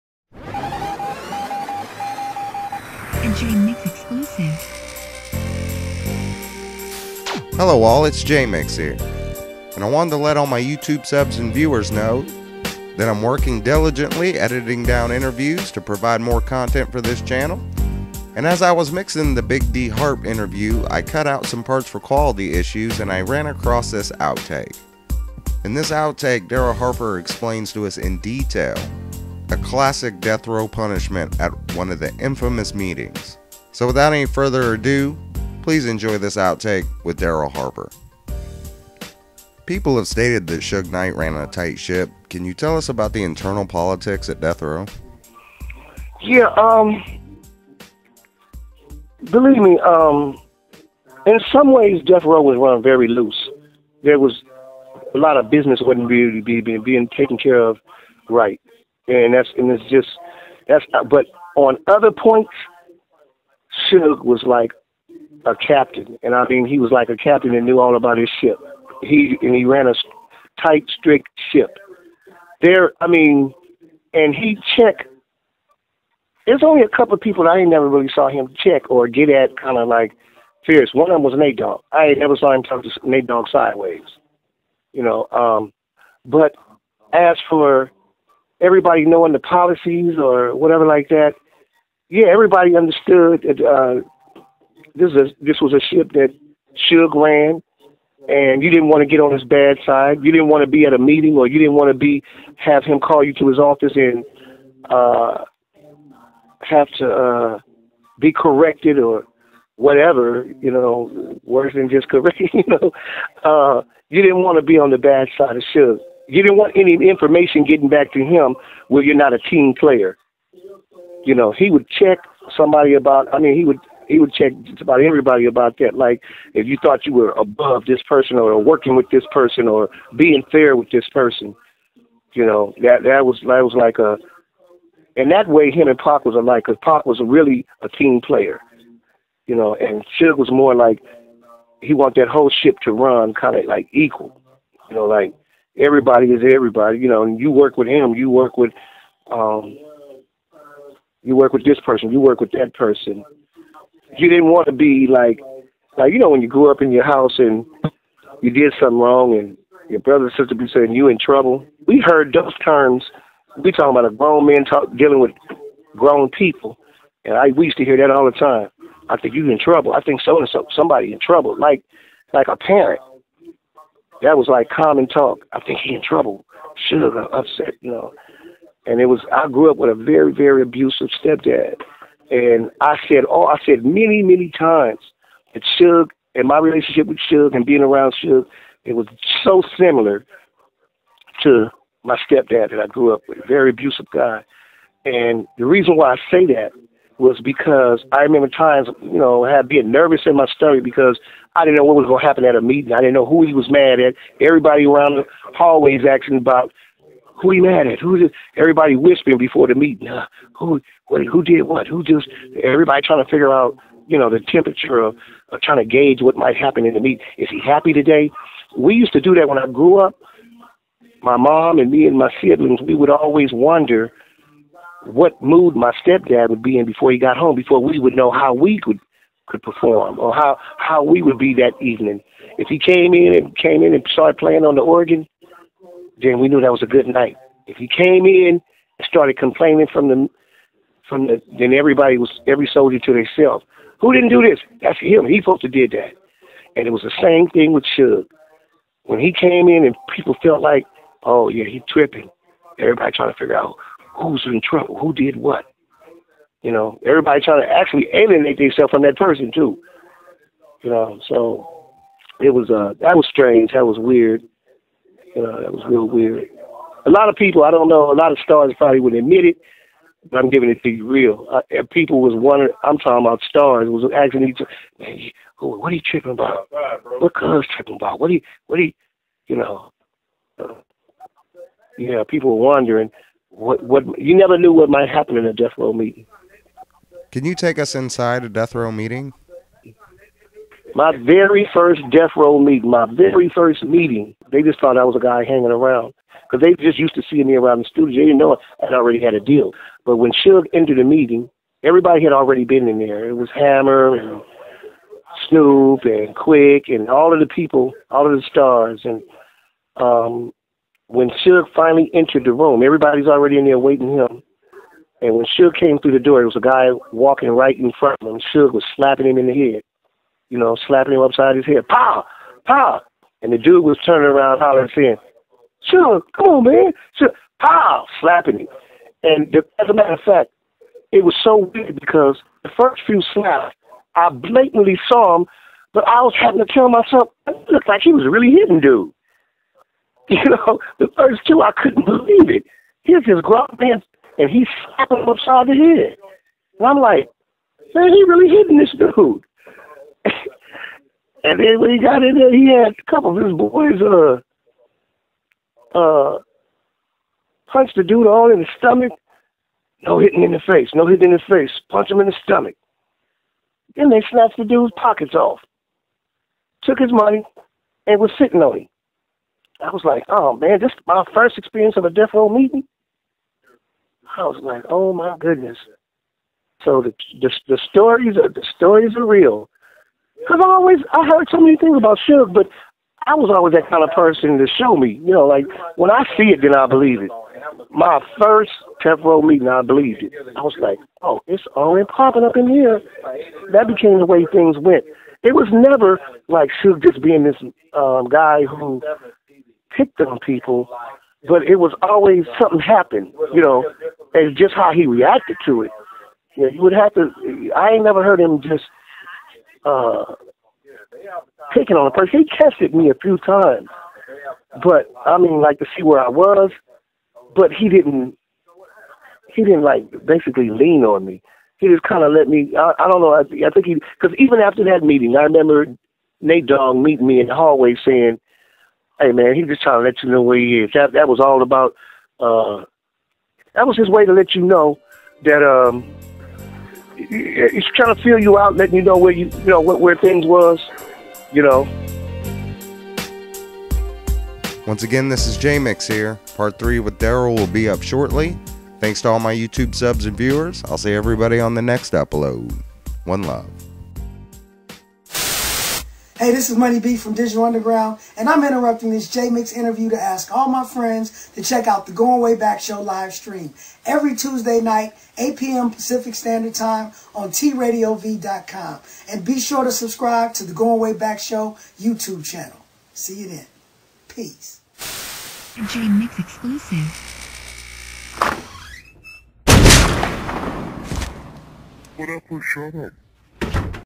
Darryl Harper Details A Death Row Beatdown With Suge Knight
JOIN TUPACNATION (LINK BELOW)
Come For The Exclusives! Stay For the Community!